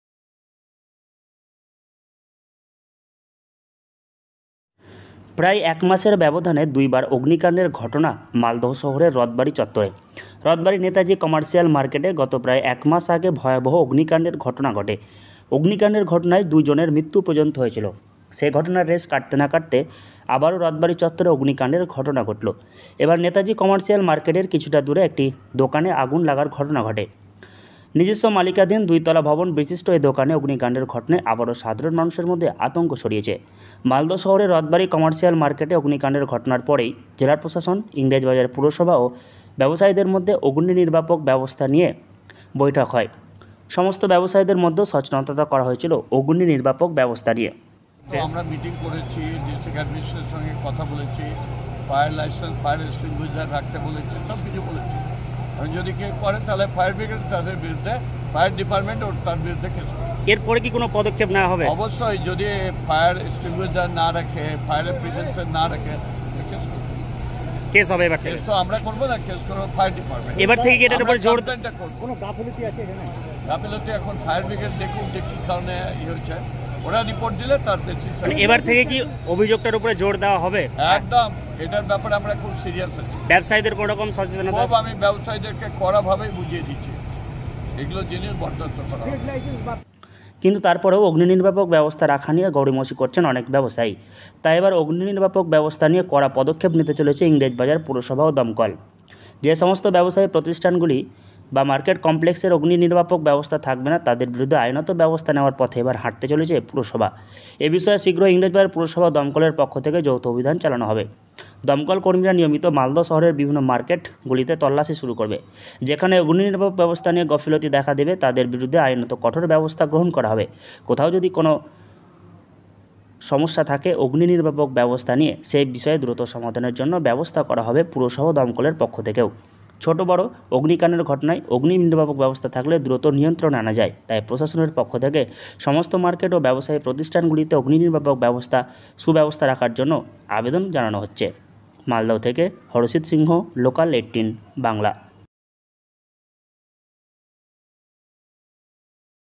Malda News: অগ্নিনির্বাপণ ব্যবস্থা বাধ্যতামূলক মালদহের ‌যে কোনও মার্কেটে! কড়া নিয়ম প্রশাসনের
মালদহ- প্রায় এক মাসের ব্যবধানে দুইবার অগ্নিকাণ্ডের ঘটনা মালদহ শহরের রথবাড়ি চত্বরে। রথবাড়ি নেতাজি কমার্শিয়াল মার্কেটে গত প্রায় একমাস আগে ভয়াবহ অগ্নিকাণ্ডের ঘটনা ঘটে। অগ্নিকাণ্ডের ঘটনায় দুইজনের মৃত্যু হয়েছিল। সেই ঘটনার রেশ কাটতে না কাটতে আবারও রথবাড়ি চত্বরে অগ্নিকাণ্ডের ঘটনা ঘটল।
এ বার নেতাজি কমার্শিয়াল মার্কেটের কিছুটা দূরে একটি দোকানে আগুন লাগার ঘটনা ঘটে। নিজস্ব মালিকাধীন দুই তলা ভবন বিশিষ্ট এই দোকানে অগ্নিকাণ্ডের ঘটনায় আবারও সাধারণ মানুষের মধ্যে আতঙ্ক ছড়িয়েছে। মালদহ শহরের রথবাড়ি কমার্শিয়াল মার্কেটে অগ্নিকাণ্ডের ঘটনার পরেই জেলা প্রশাসন ইংরেজবাজার পুরসভা ও ব্যবসায়ীদের মধ্যে অগ্নি নির্বাপনব্যবস্থা নিয়ে বৈঠক হয়। সমস্ত ব্যবসায়ীদের মধ্যেও সচেতন করা হয়েছিল অগ্নি নির্বাপনব্যবস্থা নিয়ে।